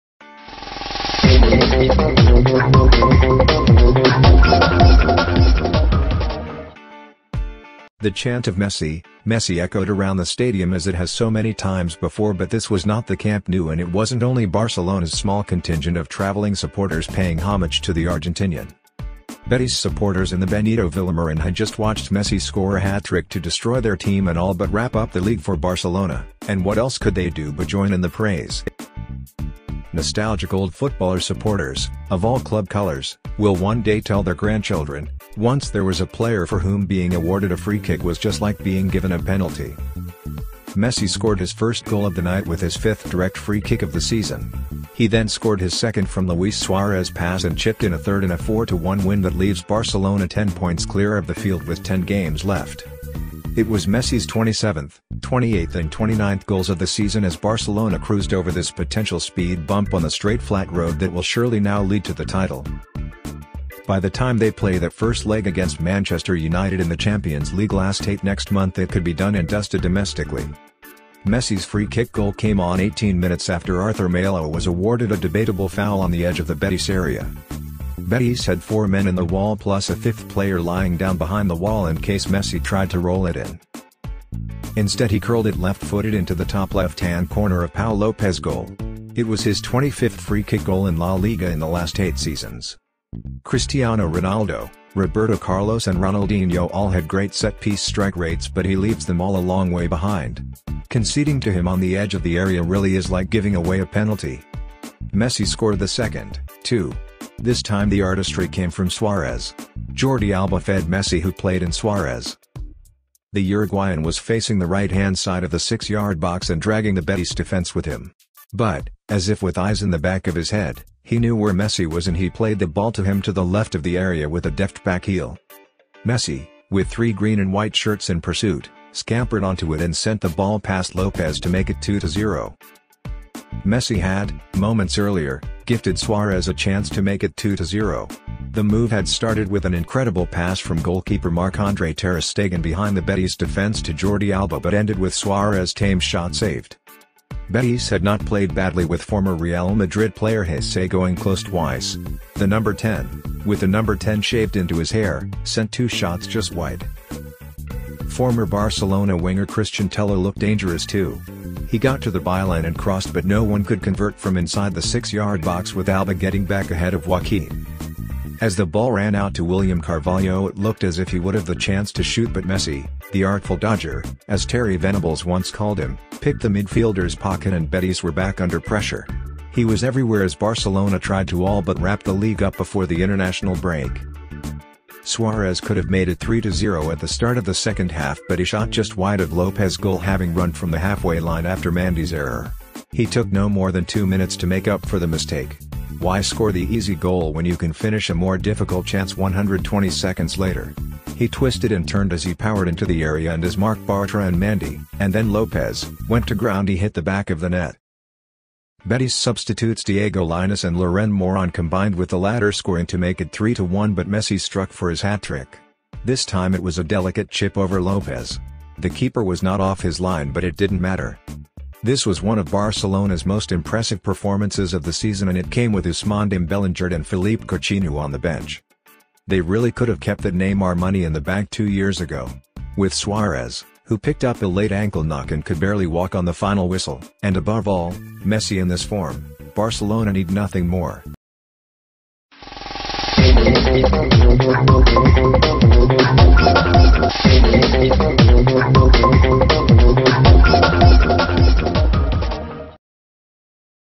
Betis fans chant Lionel Messi's name and give him a standing ovation as sublime hat-trick
The chant of 'Messi, Messi' echoed around the stadium as it has so many times before but this was not the Camp Nou and it wasn't only Barcelona's small contingent of travelling supporters paying homage to the Argentinian.

Betis supporters in the Benito Villamarin had just watched Messi score a hat-trick to destroy their team and all but wrap up the league for Barcelona, and what else could they do but join in the praise?

Nostalgic old footballer supporters, of all club colours, will one day tell their grandchildren - once there was a player for whom being awarded a free-kick was just like being given a penalty.